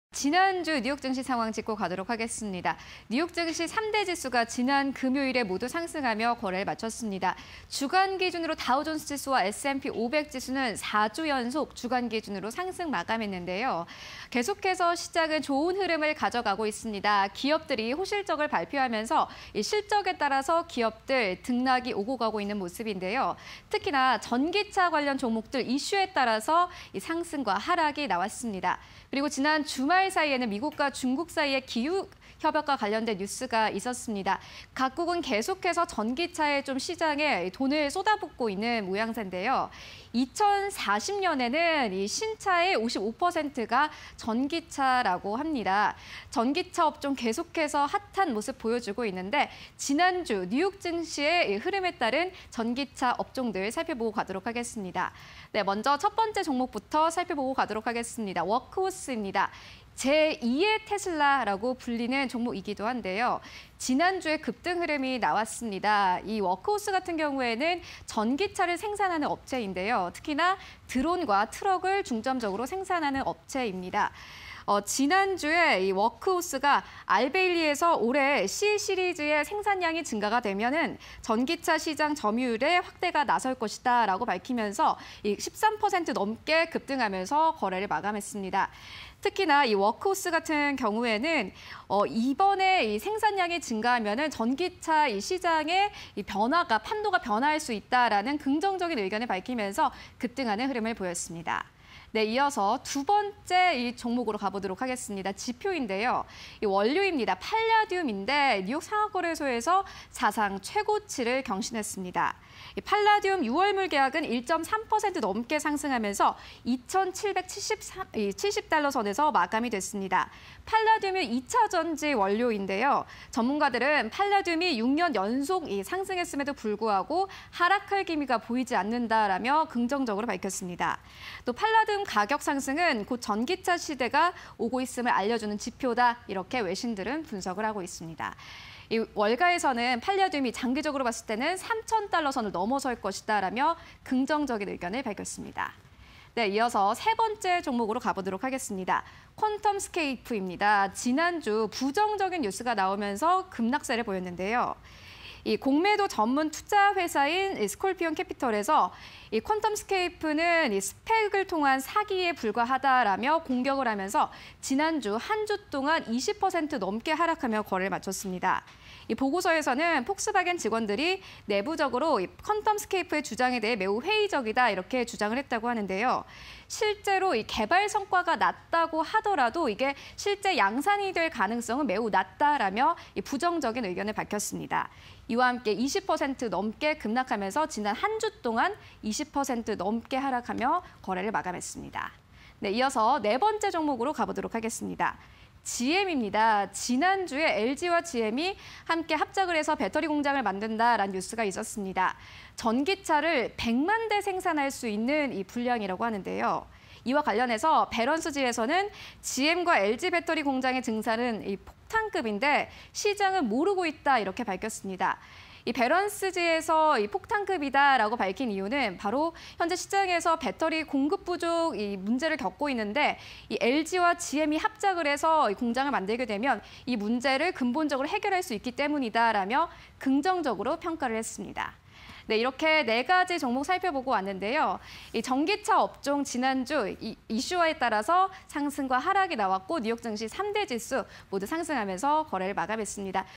제2의 테슬라 ‘워크호스’, 투자의견 상향에 13% 급등 / 팔라듐 사상 최고치… 6년째 거침없는 랠리_글로벌 마감 증시 (20210419)
- 제2의 테슬라 ‘워크호스’, 투자의견 상향에 13% 급등
- 팔라듐 사상 최고치… 6년째 거침없는 랠리
- "퀀텀스케이프, 공매도 표적 됐다"… 지난주 20%↓
- "GM·LG 배터리 공장 증설은 '폭탄급'"… 시장은 몰라